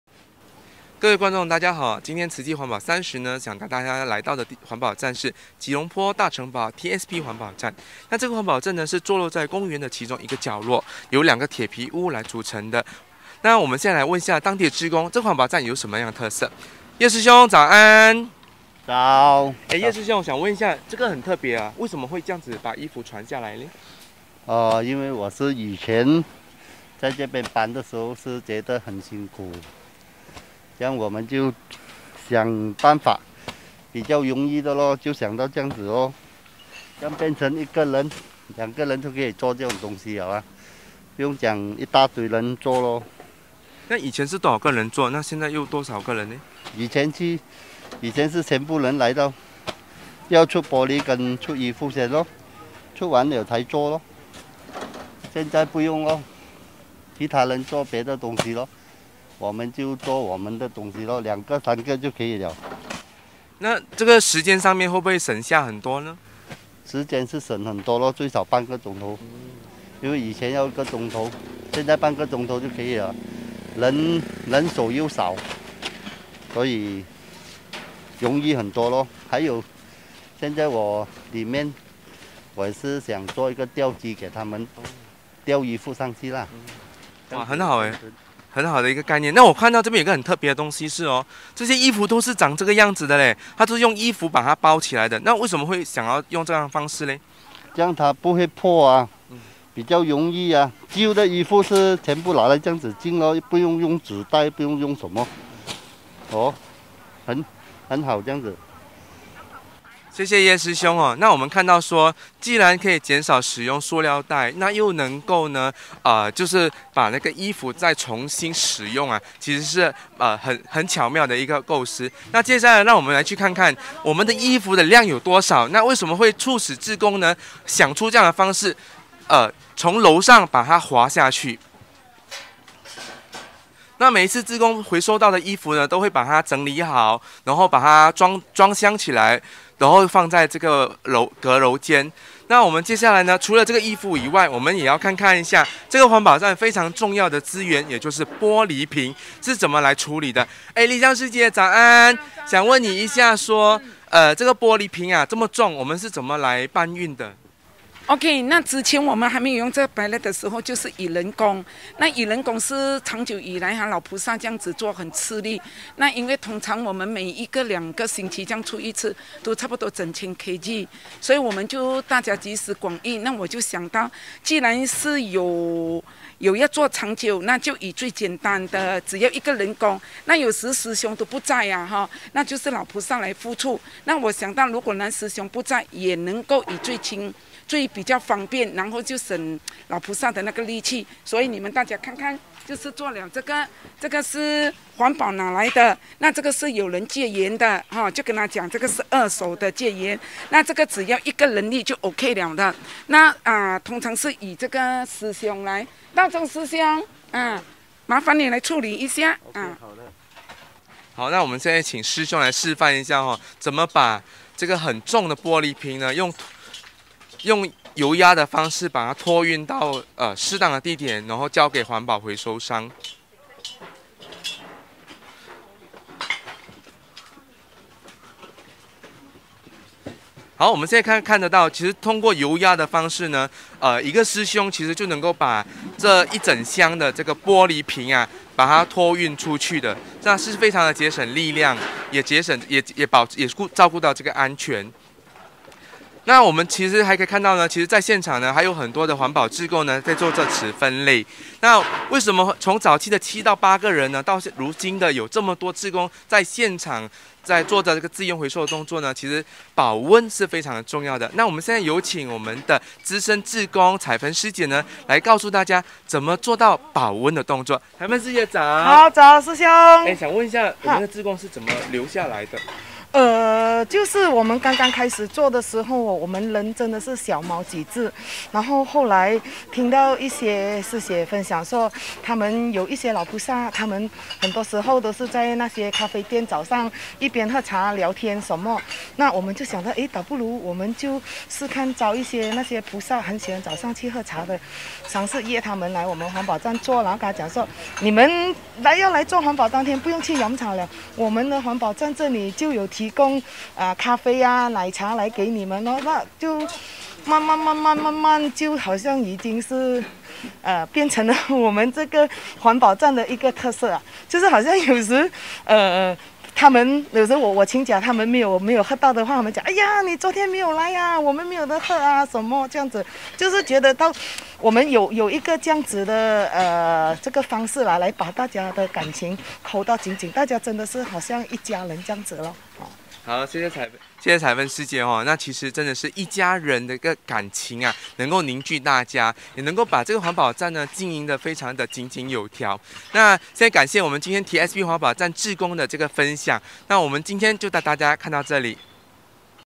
【新聞】铁皮屋环保站，回收衣溜滑梯，栈板车运玻璃，省人又省力
记者 林英意 lim eng yee
各位观众大家好
今天慈济环保三十
想带大家来到的环保站是
吉隆坡大城堡TSP环保站
这个环保站是坐落在公园的
其中一个角落
由两个铁皮屋来组成的
我们现在来问一下当地志工
这环保站有什么样的特色

慈济志工 叶芸宏 yap yoon hoong
叶师兄早安 早
叶师兄想问一下这个很特别
为什么会这样子把衣服传下来
因为我是以前在这边
搬的时候觉得很辛苦
我们就想办法比较容易的
就想到这样子
变成一人两个人
就可以做这种东西了
不用讲一大堆人
那以前是多少个人做
现在又有多少个人
以前是全部人来到
要出玻璃跟出衣服先
出完了才做
现在不用了
其他人做别的东西
我们就做我们的东西
两个三个就可以了
那这个时间上面
会不会省下很多呢
时间是省很多
最少半个钟
因为以前要一个钟头
现在半个钟头就可以了
人手又少所以容易很多
还有现在我里面
我也是想做一个吊机给他们
吊衣服上去
很好的一个概念
那我看到这边
有一个很特别的东西是
这些衣服都是长这个样子的
他都用衣服把它包起来的
那为什么会想要用这样方式
这样它不会破 比较容易
旧的衣服全部拿来这样子进
不用用纸袋 不用用什么
很好这样子
谢谢叶师兄那我们看到说
既然可以减少使用塑料袋
那又能够就是
把那个衣服再重新使用
其实是很巧妙的构思
接下来让我们去看看
我们的衣服的量有多少
为什么会促使志工
想出这样的方式
从楼上把它滑下去
那每一次志工回收到的衣服
都会帮他整理好
然后把它装装箱起来
然后放在这个阁楼间
那我们接下来除了衣服以外
我们也要看看一下
环保站非常重要的资源
也就是玻璃瓶是怎么来处理的
丽香师姐早安
想问你一下说
这个玻璃瓶这么重
我们是怎么来搬运的

侧标2:
栈板车运玻璃
省人又省力

慈济志工 陈丽香 Tan Lay Heong
那之前我们还没用
这个木栈板的时候
就是以人工（搬）
 以人工是长久以来
老菩萨这样子做很吃力
那因为通常我们每一个
两个星期这样出一次
都差不多整千公斤
所以我们就大家集思广益
那我就想到既然是有要做长久
那就以最简单的
只要一个人工
有时师兄都不在
那就是老菩萨来付出
那我想到如果师兄不在
也能够以最轻最比较方便
然后就省老菩萨的那个力气
所以你们大家看看
就是做了这个
这个就是环保拿来的
那这个是有人结缘的
就跟他讲这个是二手的结缘的
这个只要一个人力就ok了
通常是以这个师兄来
道忠师兄
麻烦你来处理一下
我们现在请师兄来示范一下
怎么把这个很重的玻璃瓶
用用油压的方式
把它托运到适当的地点
然后交给环保回收商
我们现在看得到
其实通过油压的方式
一个师兄其实就能够把
这一整箱的玻璃瓶
把他拖运出去的
是非常的节省力量
也节省也照顾到安全
那我们其实还可以看到
其实在现场还有很多的
环保志工做着纸分类
为什么从早期的七到八个人
到如今的有这么多志工
在现场在做资源回收的动作
其实保温是非常的重要的
那我们现在有请
我们的资深志工
彩芬师姐来告诉大家
怎么做到保温的动作

侧标3: 奶茶咖啡飘香扣感情

慈济志工 黄彩芬Ooi Chai Feng
彩芬师姐早 早师兄
想问一下你们的志工
是怎么留下来的
就是我们刚刚开始做的时候
我们人真的是小猫几只
后来听到一些师姐分享说
他们有一些老菩萨
他们很多时候
都是在那些咖啡店
早上一边喝茶聊天什么
那我们就想说到不如
我们就试看招一些那些菩萨
很喜欢早上去喝茶的
尝试约他们来我们环保站做
然后跟他们说你们要来做环保
当天不用去喝茶了
我们的环保站这里就有提供
咖啡奶茶来给你们
慢慢慢慢慢慢就好像已经是
变成我们环保站的一个特色
就好像有时他们有时候我请假
他们没有没有喝到的话
哎呀你昨天没有来呀
我们没有得喝什么这样子
就是觉得我们有一个这样子的
方式来把大家的感情扣到紧紧
大家真的是好像一家人这样
谢谢彩芬师姐
那其实真的是一家人的感情
能够凝聚大家
也能够把这个环保站
经营得非常的井井有条
感谢我们今天
TSP环保站志工的分享
我们今天就带大家看到这里